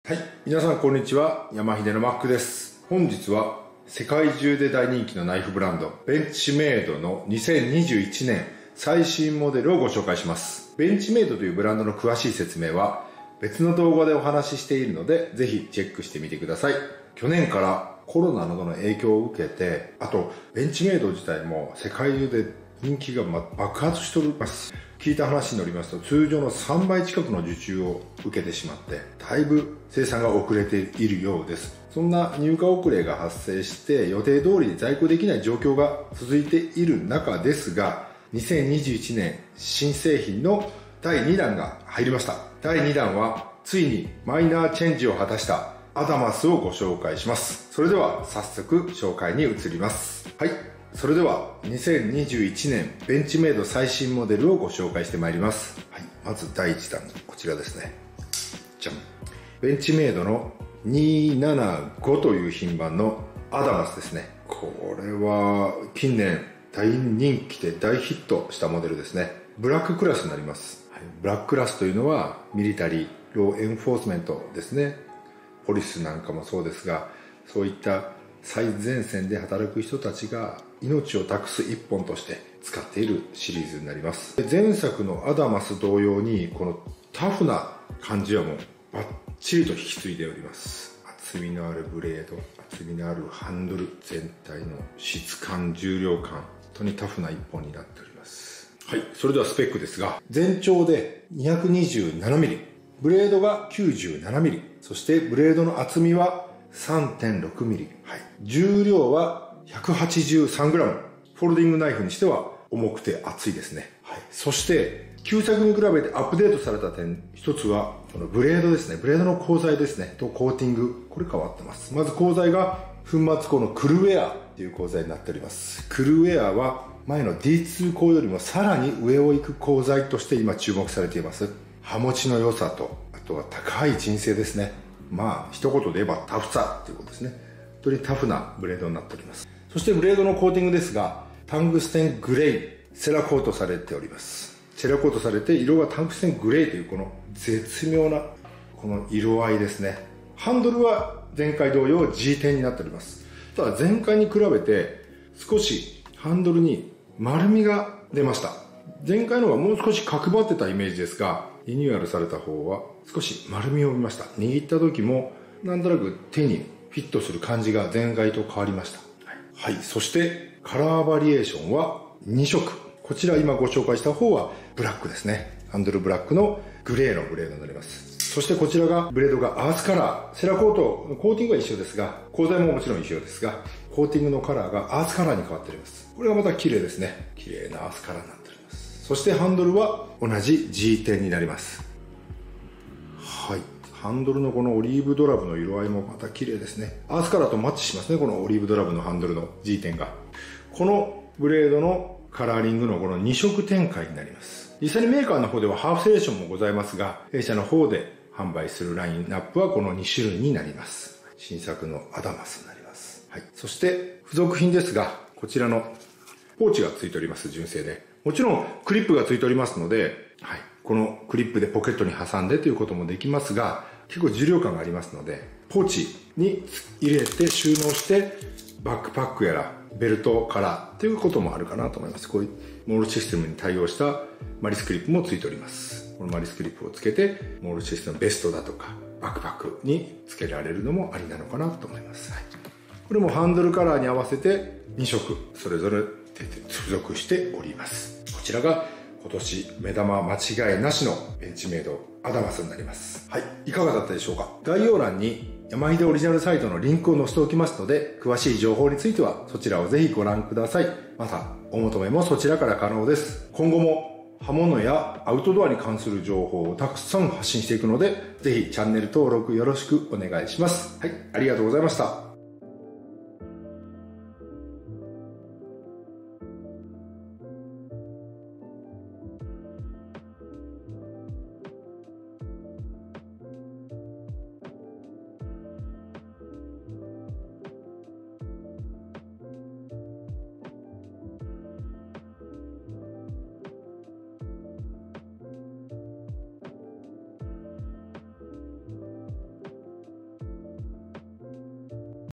老舗刃物屋のプロが解説！世界一売れているBENCHMADE(ベンチメイド)の最新モデル"Adamas(アダマス)"を徹底解説！
世界が認めるNo.1ナイフブランド、"BENCHMADE(ベンチメイド)"の2021年最新モデル、ブラッククラスの中でもトップクラスの人気を誇ったアダマスがマイナーチェンジを果たし今年再登場！
その全貌を公開！
全世界がクールに生まれ変わったこのタフな奴を待っていました！